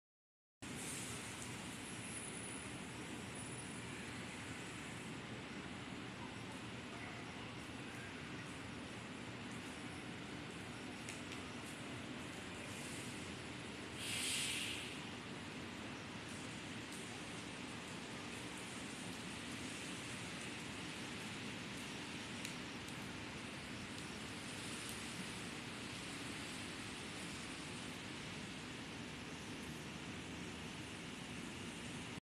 遲遲不通車的機場捷運-桃園高鐵站 | 自由趴趴走 | 悠遊台灣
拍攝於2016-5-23
請大家一同見證下次拍攝是否通車了